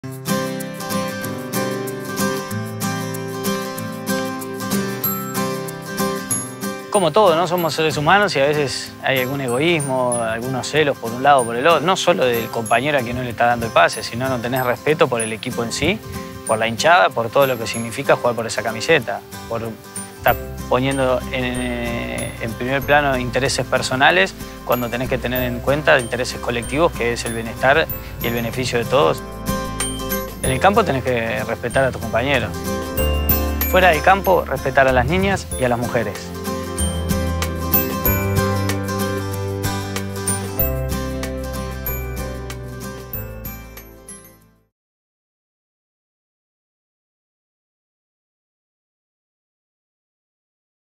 Session 2 : Forlan for “FIFA 11 for Health"
Uruguay magic man Diego Forlan talks about keeping your team-mates involved and respecting women and girls.